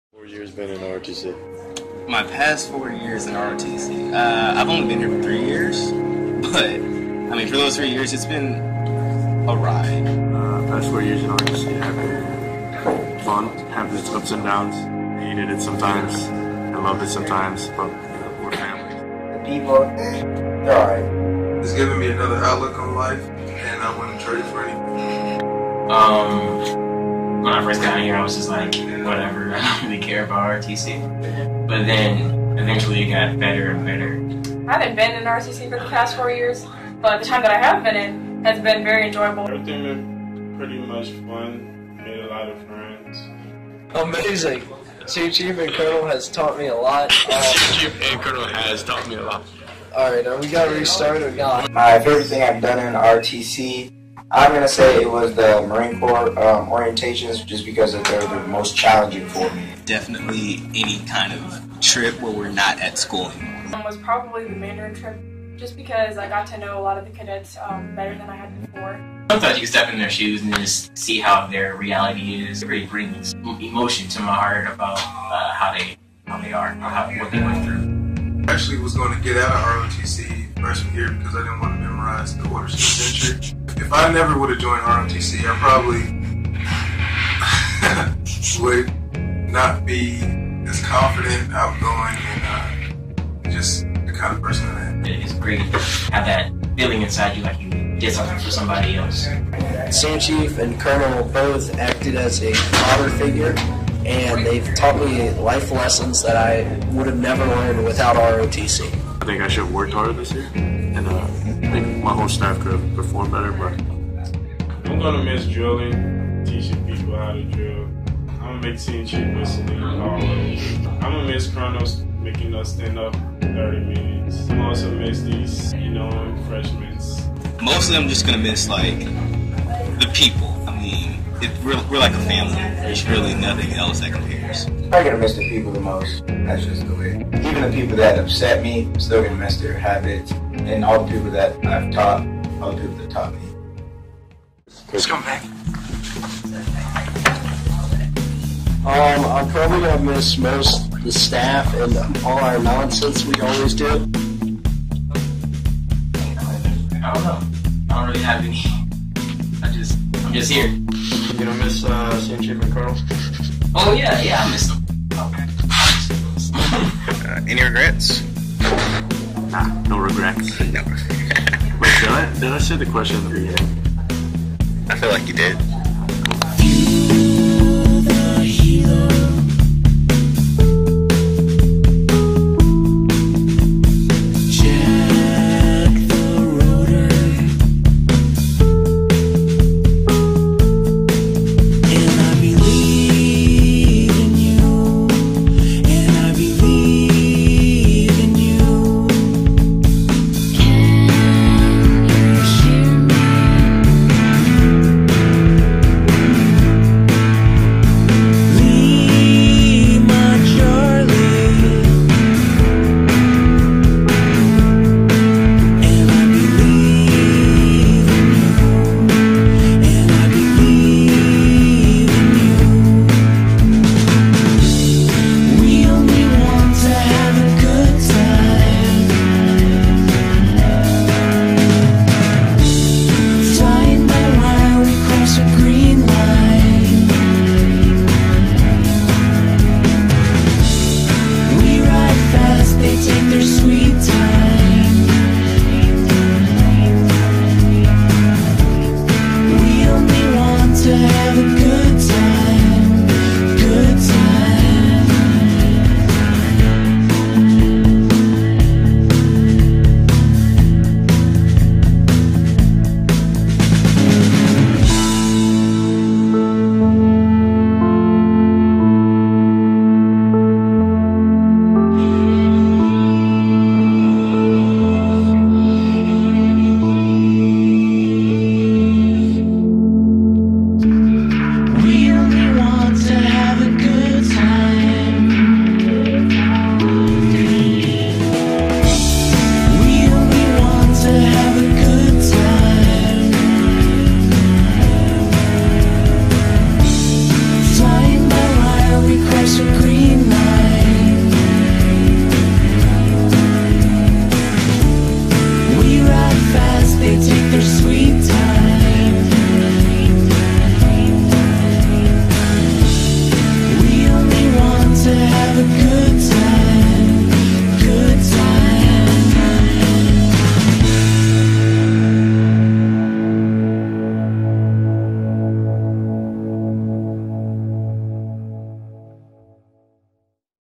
Final Goodbye - Druid Hills High School NJROTC Class of 2015
This is a video created by the Druid Hills NJROTC Class of 2015. I (PAO) along with the other the unit ,with consent from our instructor, agree that this video shine light on the reality of JROTC and it should be available to moviate for anyone that is thinking about joining JROTC.

If you would like to get in to contract with the Unit please contract us at :